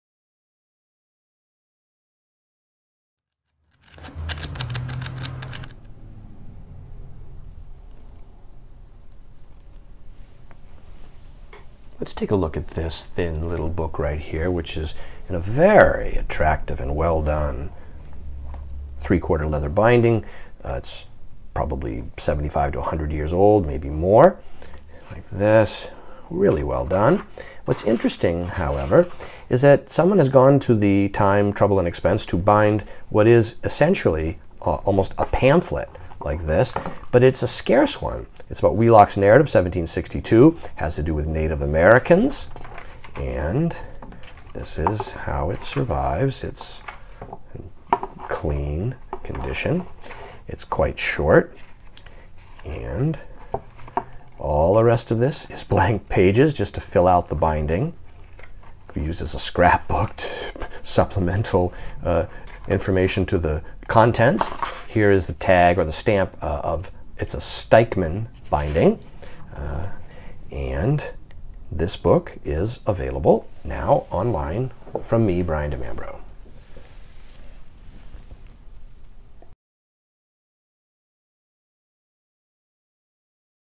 Stikeman fine leather bound book c.1900 Wheelock's Narrative Indian Charity School 1762 Lebanon CT
Wheelock's Narrative of Indian Charity School [Lebanon, Connecticut]: or, A Plain and Faithful Narrative, by Eleazar Wheelock.
Published c.1890-1900. 20 pages. Early 3/4 leather binding, stamped by Stikeman & Co.

A 100 year old reprinting of Eleazar Wheelock's "A Plain and Faithful Narrative", a pamphlet first published in 1763, which details the progress of Wheelock's charity school. The school was originally founded to provide Christian education to the indigenous population then inhabiting New England, eventually leading to Dartmouth.

see: Evans, American Imprints, 9537 [printed 1763] and Field, Indian Bibliography, 1.

Book measures: 7 7/8" H X 5 1/2" W.

[V1826].